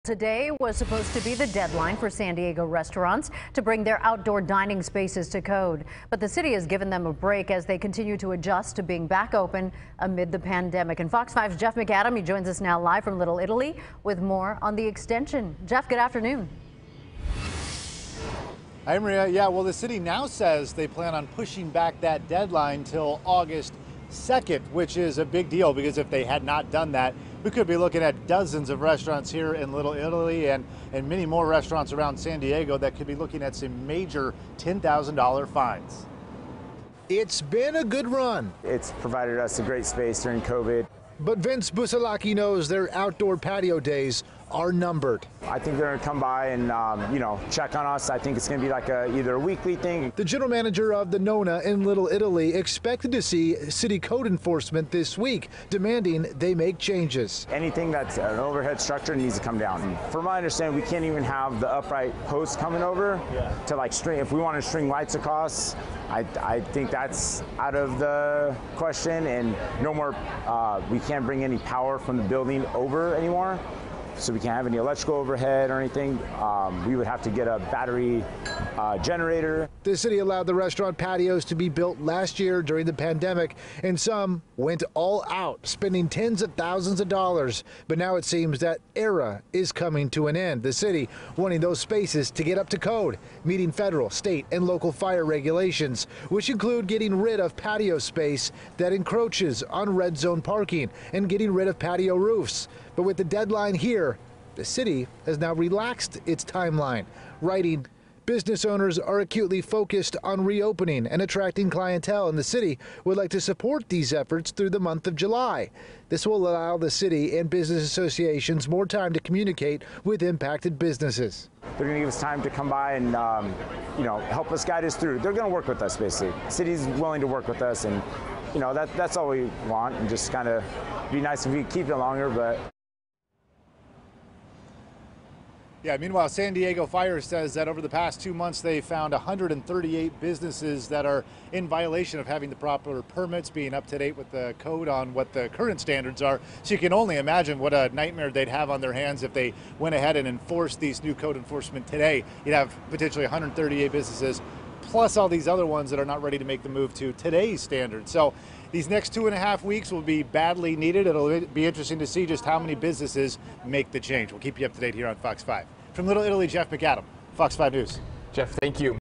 Deadline Extended To Fix Outdoor Dining Spaces
Restaurants in San Diego are facing a new deadline in order to keep their outdoor dining spaces open. A city spokesperson said Tuesday morning that the deadline has been extended to Aug. 2.